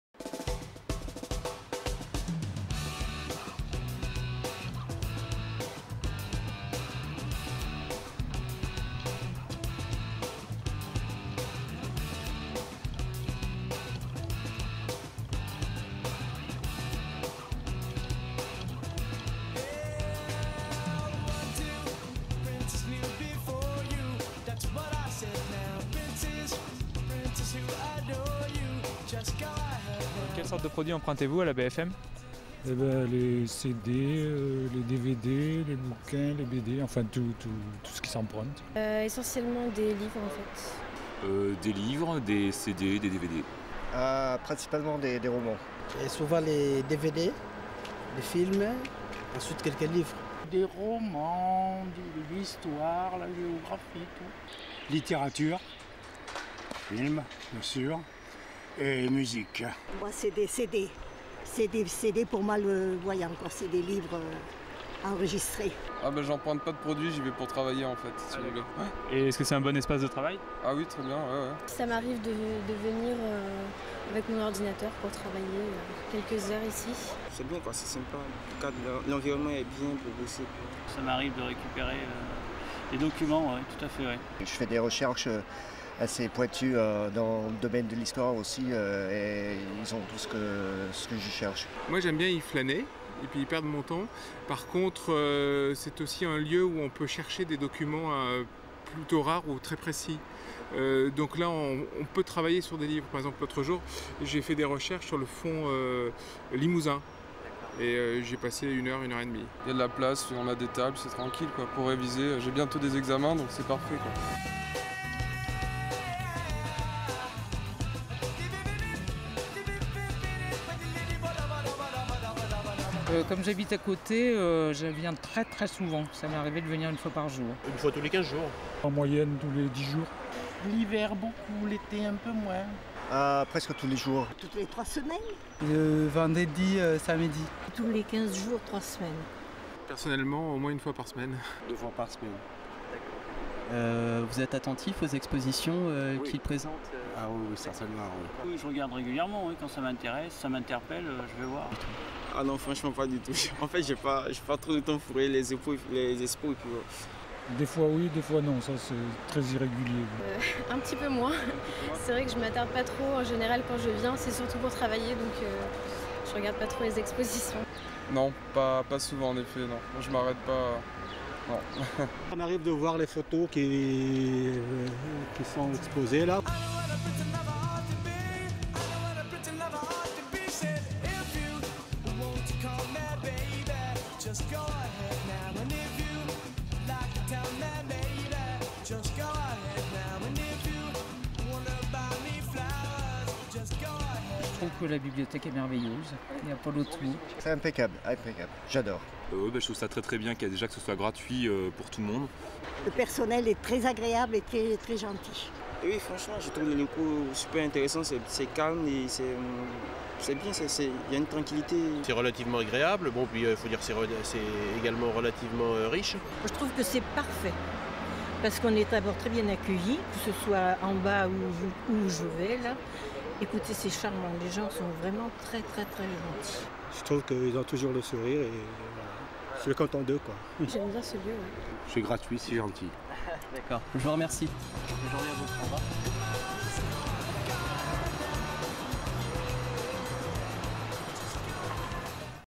Bfm: Paroles d'utilisateurs
Structurée autour de grands pôles thématiques, la Bfm centre-ville de Limoges est, avec les cinq bibliothèques de quartiers, mais également avec la bibliothèque du Conservatoire national de région et le centre de documentation sur l'émail du musée municipal, la tête de pont d'un véritable réseau de lecture publique.